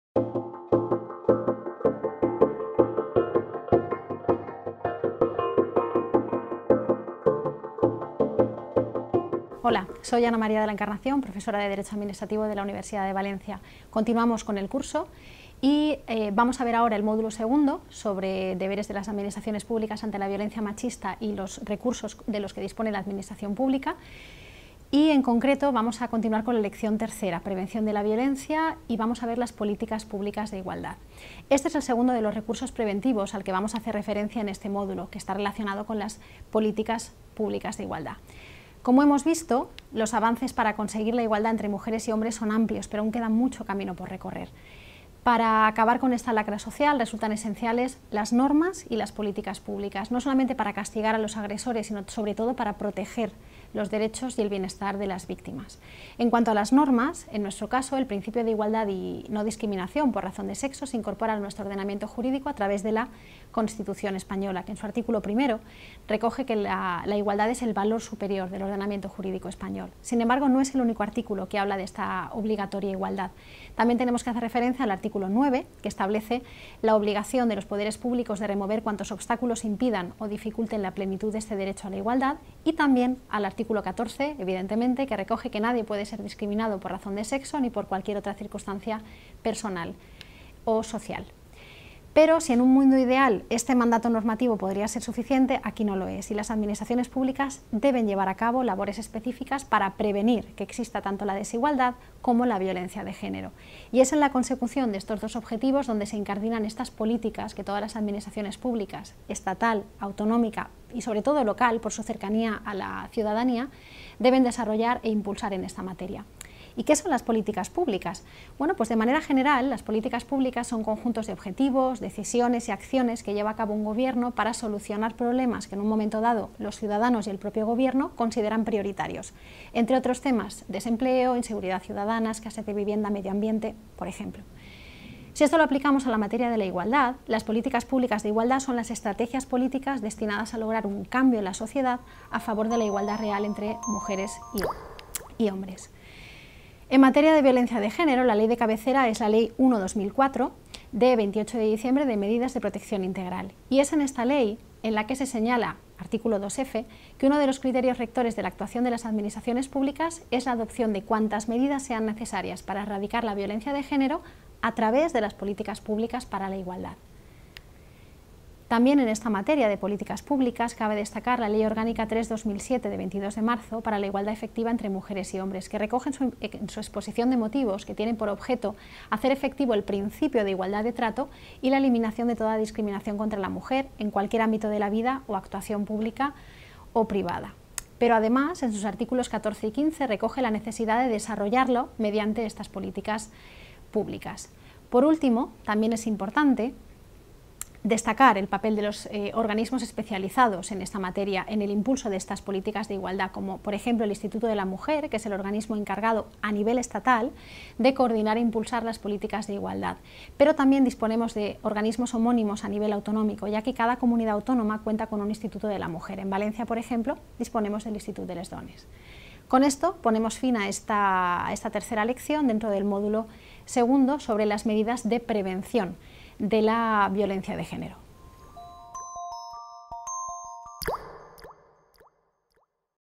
3.2.3. Prevención de la Violencia (III): Políticas públicas de igualdad
Autora: De la Encarnación Valcárcel, Ana María;
Serie: Formación básica en materia de violencia de género
Fecha: 2019
Resumen: En este video se explican las medidas del públicas en materia de igualdad.